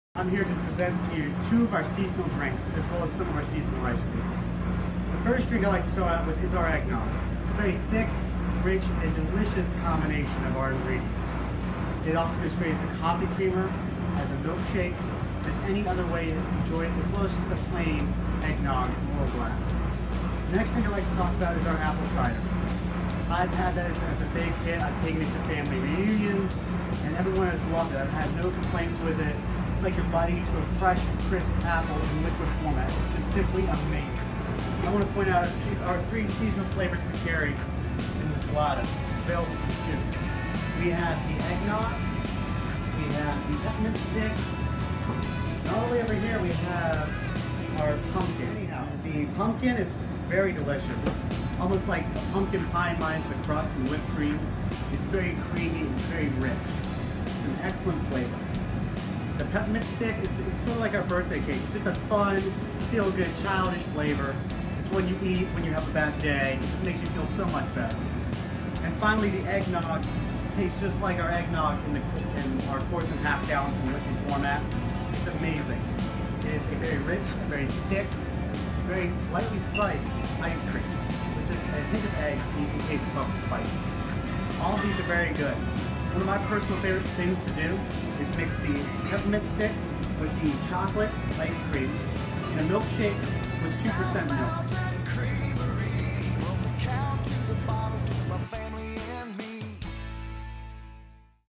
South Mountain Creamery Store Highlights!
Meet Joe! At South Mountain Creamery, we love providing our customers with delicious foods of every kind, especially those that use our fresh from the farm milk. And who doesn't like ice cream? We had Joe taste test all our holiday flavor items and give his official review. Be sure to come try them for yourself at Karen's Kountry Store, open from 10a-6pm!